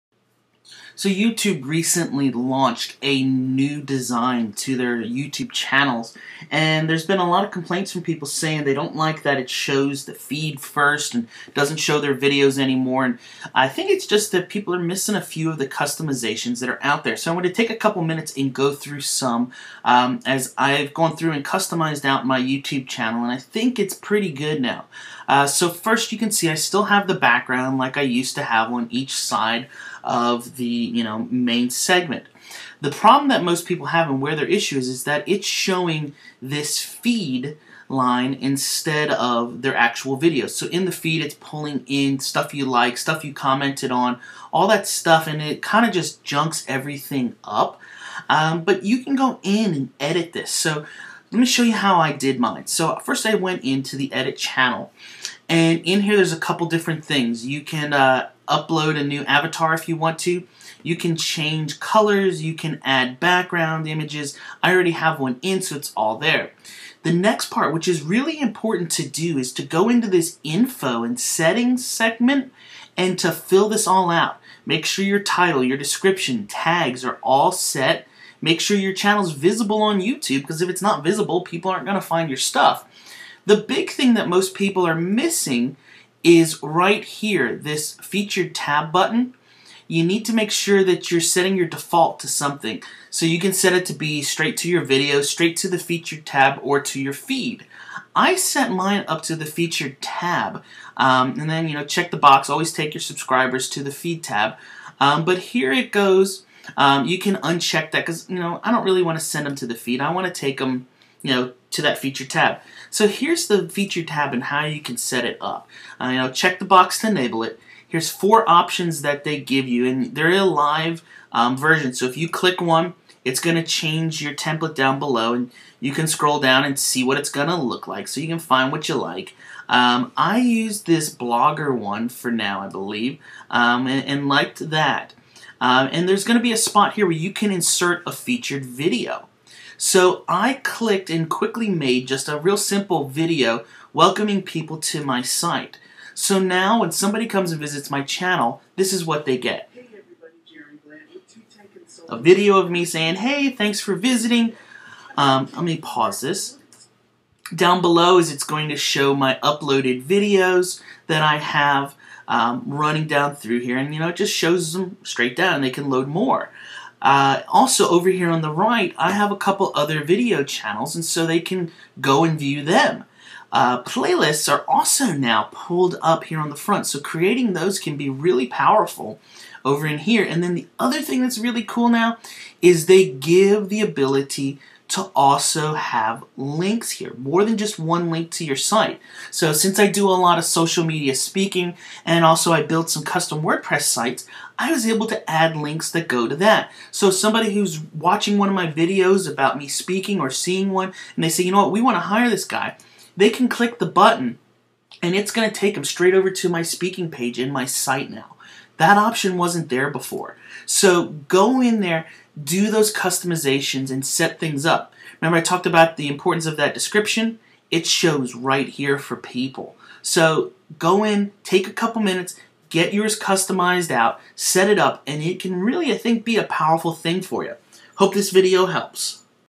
How to Customize New YouTube Channel Design
How to customize new YouTube Channel.  YouTube released a new design for YouTube channels.  Here is a quick tutorial to help you customize your new YouTube Channel.

For more information on how to customize the new YouTube Channel,